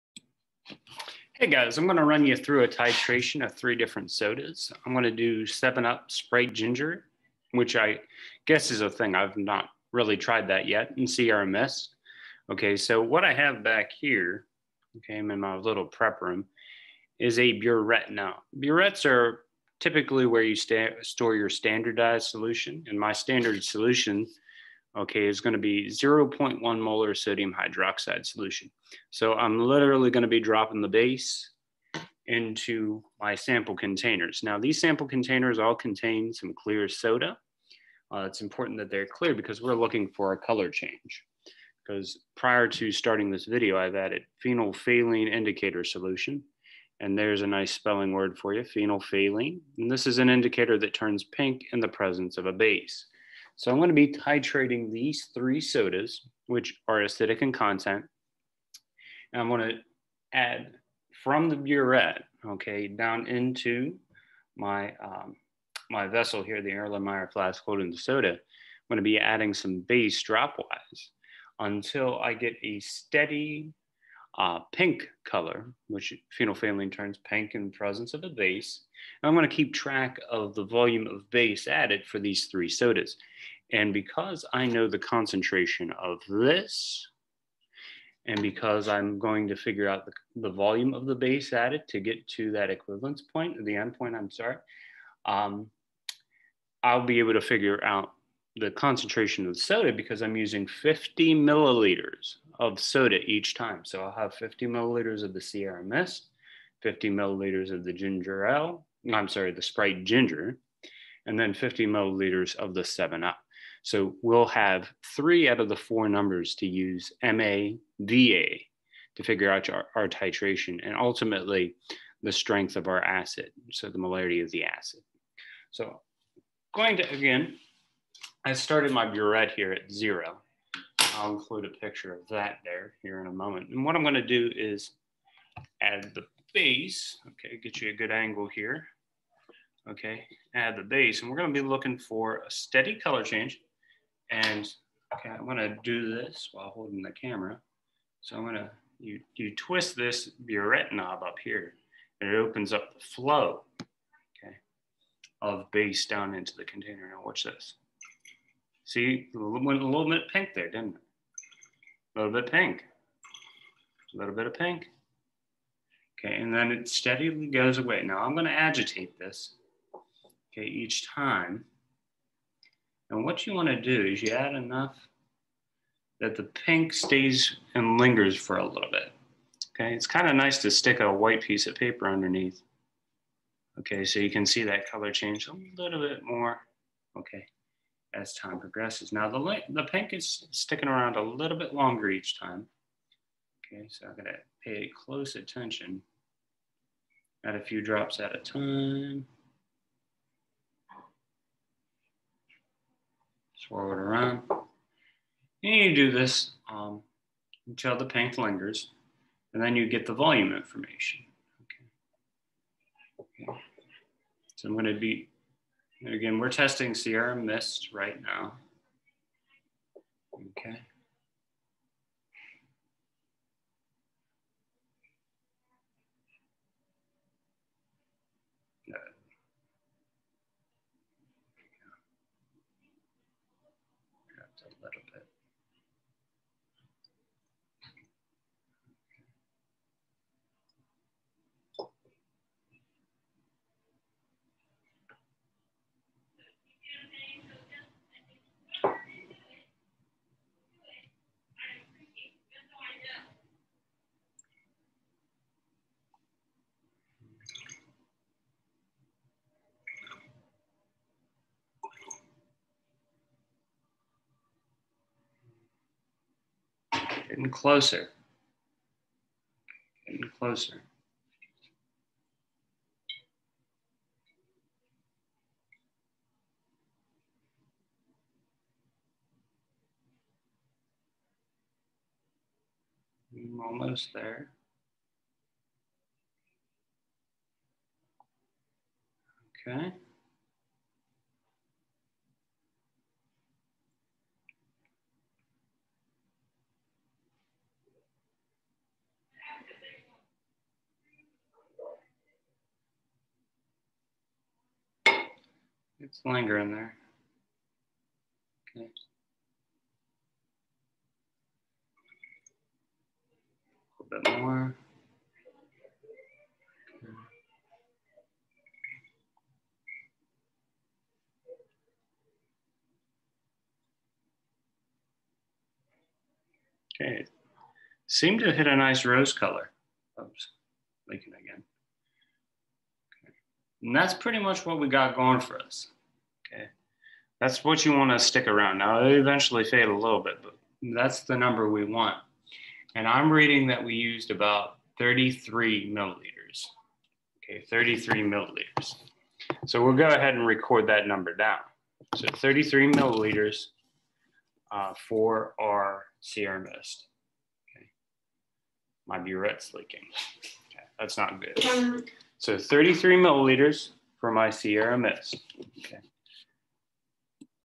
Titration of a Soda
Watch the titration of 50-mL Sierra Mist with 0.1 M sodium hydroxide solution